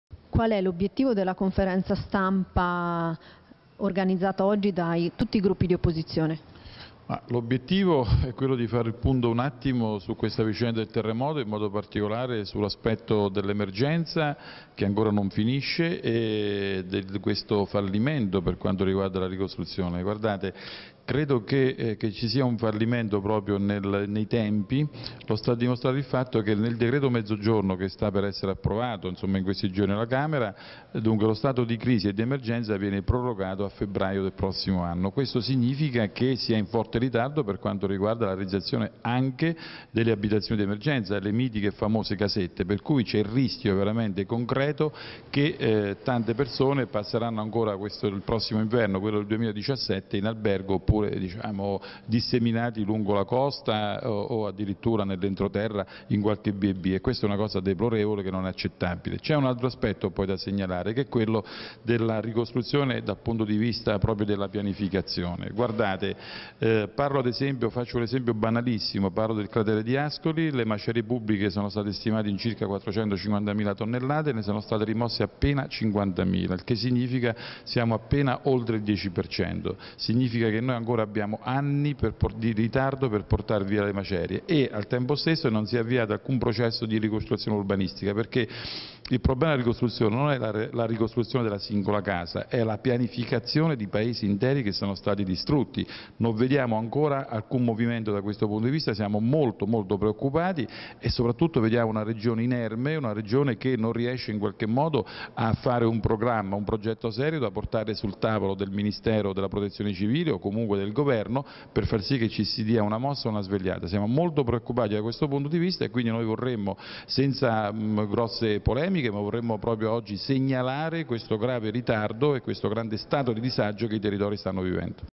Intervista a Piero Celani - Consigliere Regione Marche
Piero Celani - Consigliere Regionale di Forza Italia commenta gli esiti di una conferenza stampa con tutte le componenti politiche di opposizione presenti nel Consiglio Regionale delle Marche. Dall'incontro emergono criticità sulle attività di ricostruzione post sisma.